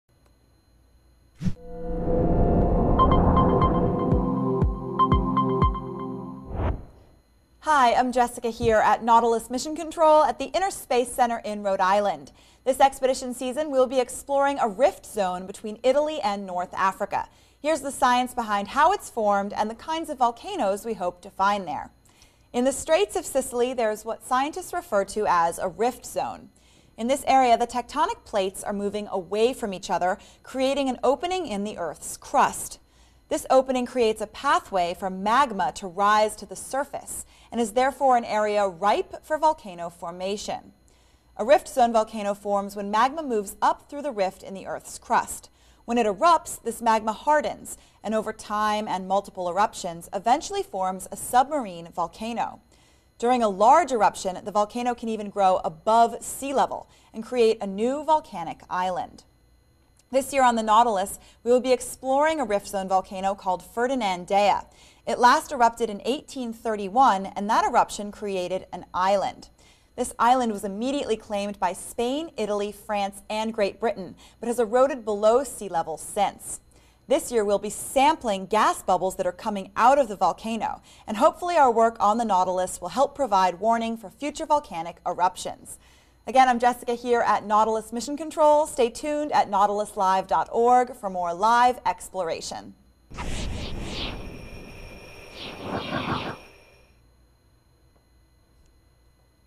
BEHIND THE SCIENCE 2011 | Rift Zone Volcano
Explore the science behind rift zone volcanoes, learn how they form, and why studying them is important.